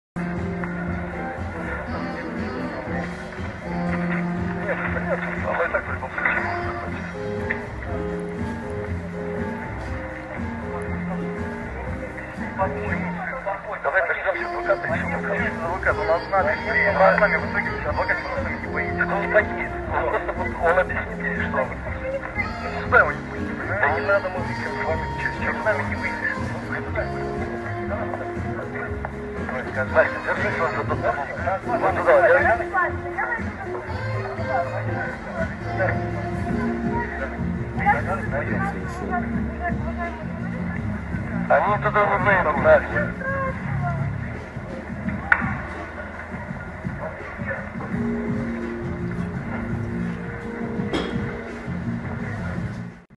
Настя Рыбка превратила в балаган свое задержание
Депортированная из Таиланда Анастасия Вашукевич, более известная как Настя Рыбка, попыталась оказать сопротивление российским оперативникам по прибытии в Москву. Снятое на видео противостояние получилось недолгим.